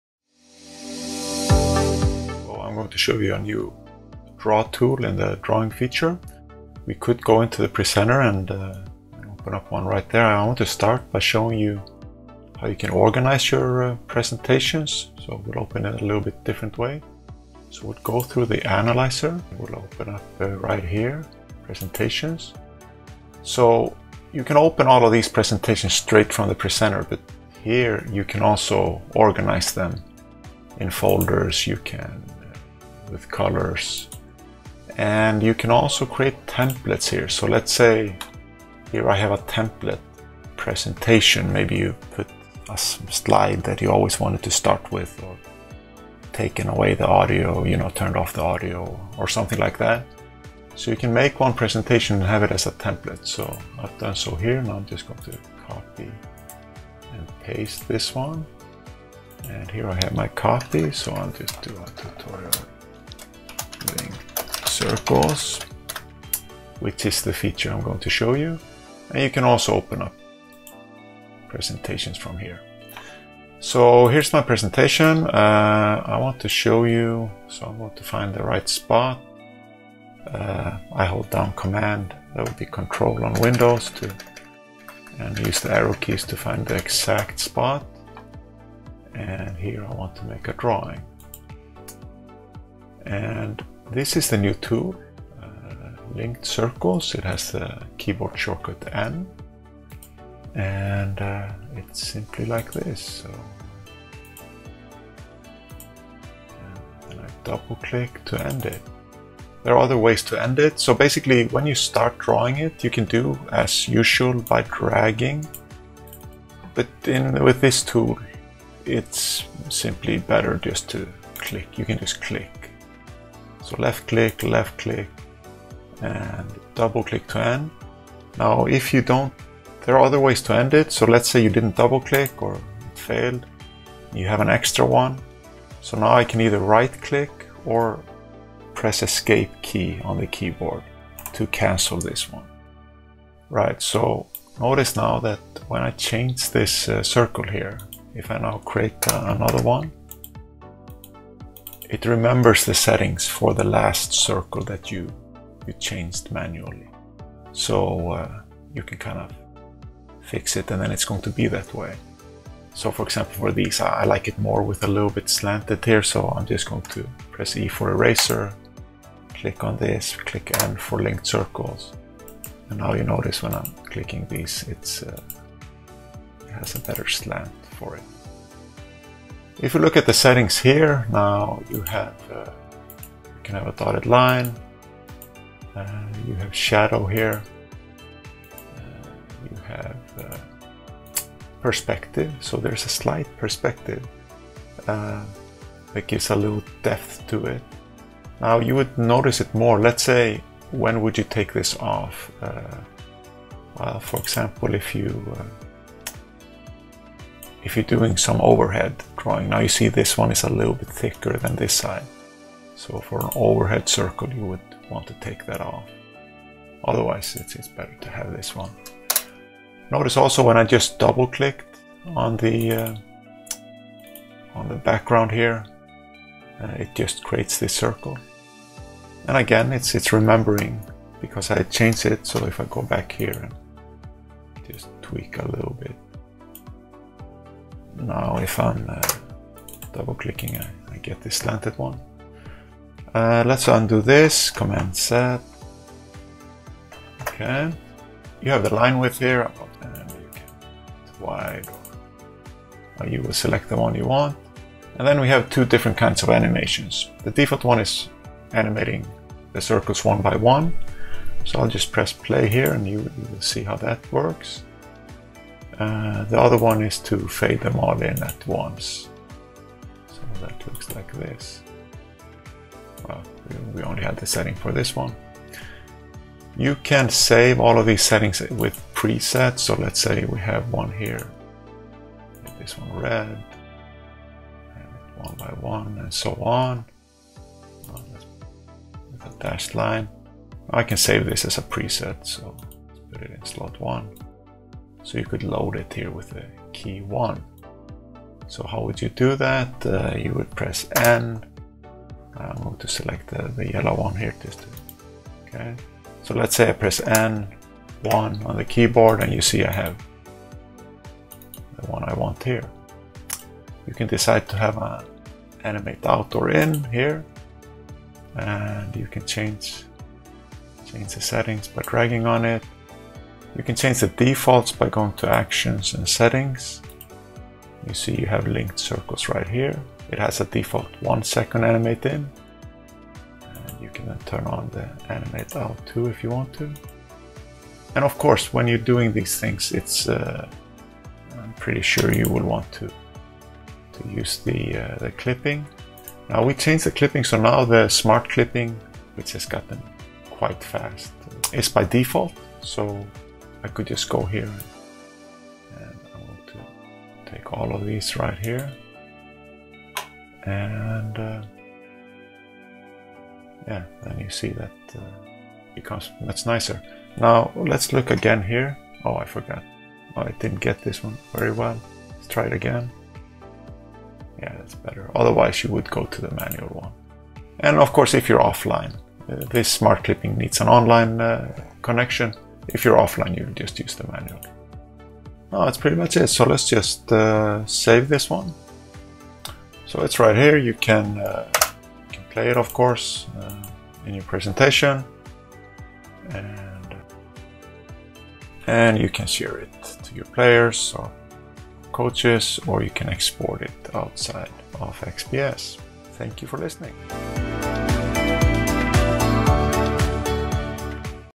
Linked Circles | Analyzer Drawing Tools | XPS Courses & Tutorials
Check out the newest addition to the Analyzer and Presenter drawing tools! You can now use animated linked circles to highlight a structure or a group of players!

0:12 | Organising presentations in Analyzer
1:12 | Linked Circles drawing tool
2:57 | Perspective, shadow, dotted line, animations
5:18 | Changing default settings of the tool
5:42 | Smart clipping